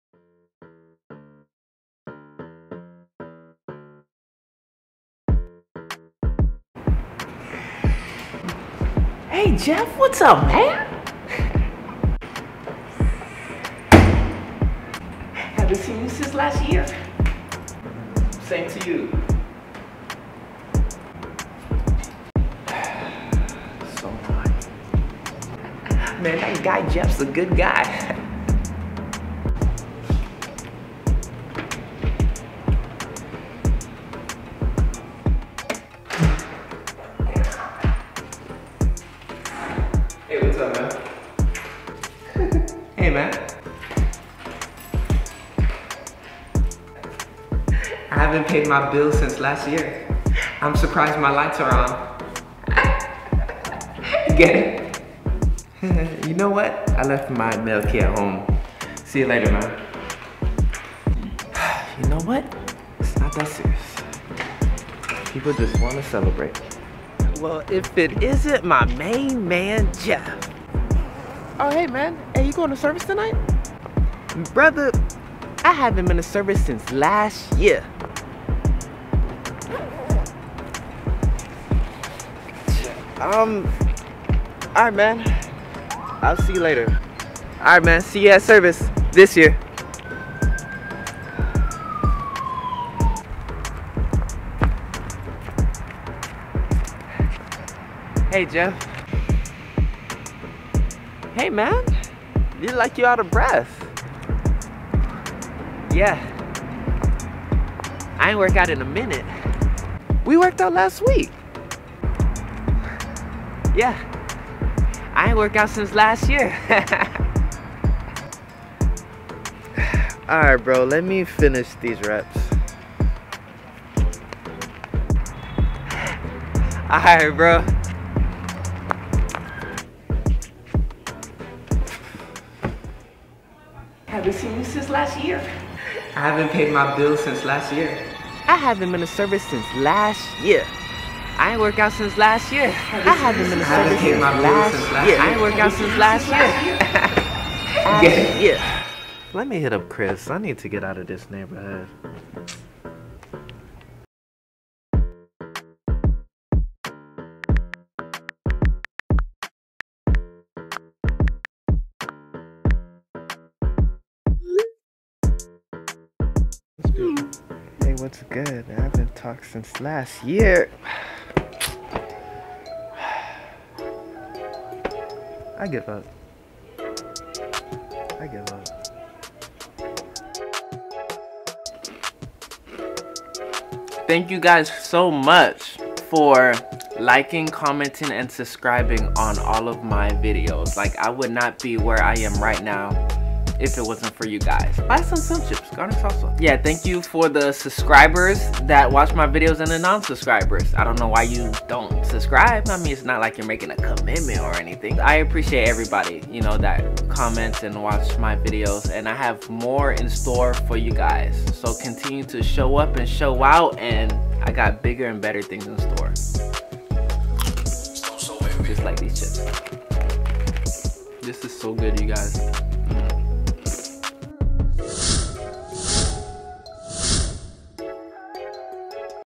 Annoying New Year's Jokes | Comedy skit
You ever get tired of hearing the same jokes every year?

Written: Jeff the Artist

Edited/ Directed: Jeff the Artist/ Drika Will

Filmed: Drika Will